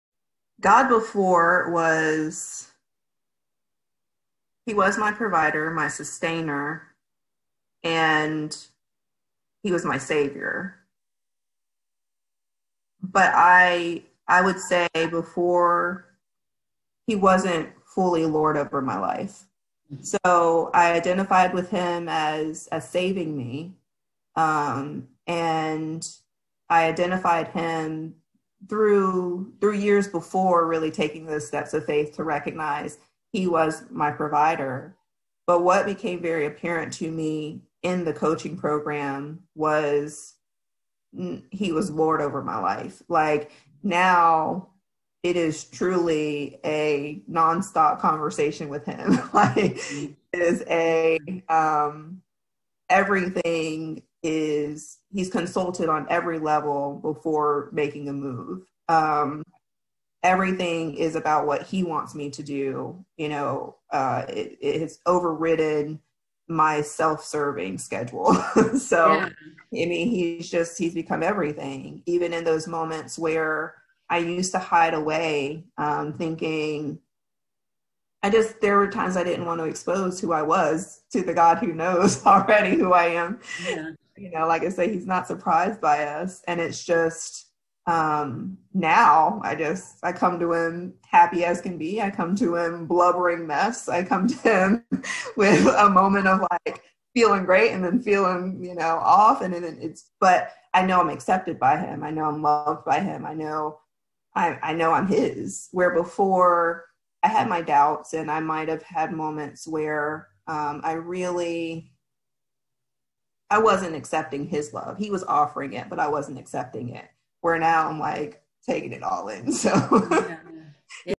My Savior vs. My Lord
Tiffiany Langston's graduating interview

Millennial Digital Ministry Academy